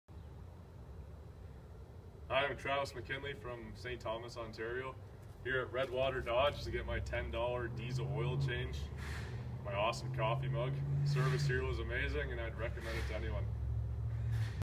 Travis from St. Thomas, Ontario | $10 Oil Change
Travis Mckinlay came into today from St. Thomas, Ontario, working in Alberta 2 hours north of Grande Prairie and drove here because of not only the $10 diesel oil change but of the knowledge of parts/labour quote for changing both fuel filters/tire rotation/air filter ​​over the phone from the service department when other dealerships were unable to perform the same quality of service. Travis was super impressed with the service department and the friendly atmosphere of the sales team and Tracey.

He will definitely be back in the future!

For full details on our $10 Oil Change Program

Redwater Dodge
4716 48 th Ave
Redwater, Alberta
T0A 2W0